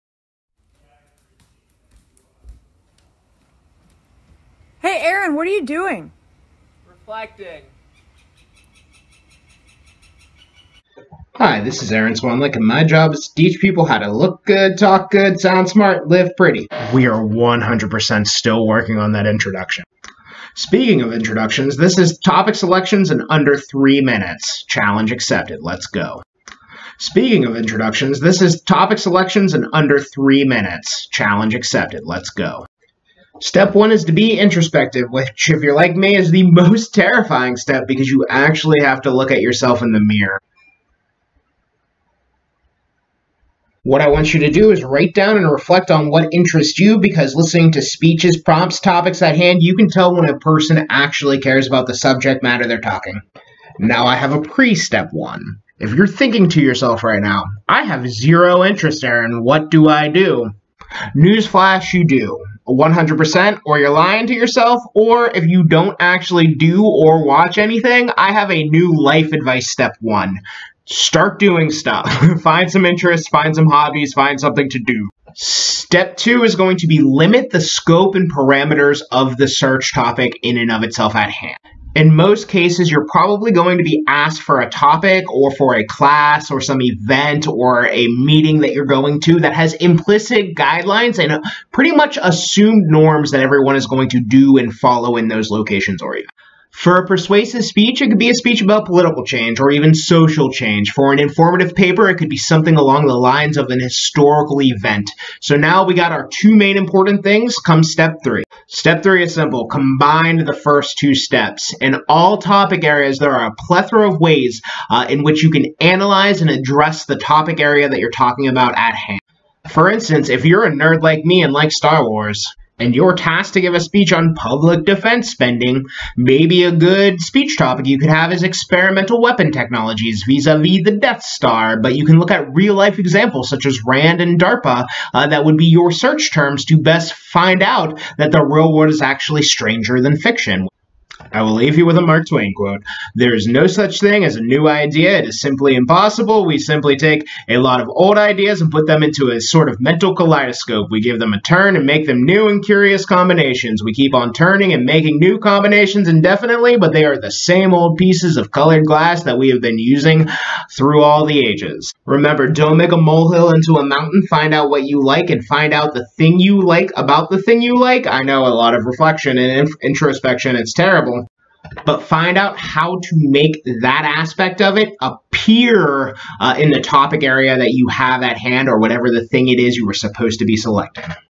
Comm 107 - Topic Selection - Challenge Mode
I thought it would be a fun idea to try to make this video in three minutes or less as a challenge. In hindsight I should have made it four.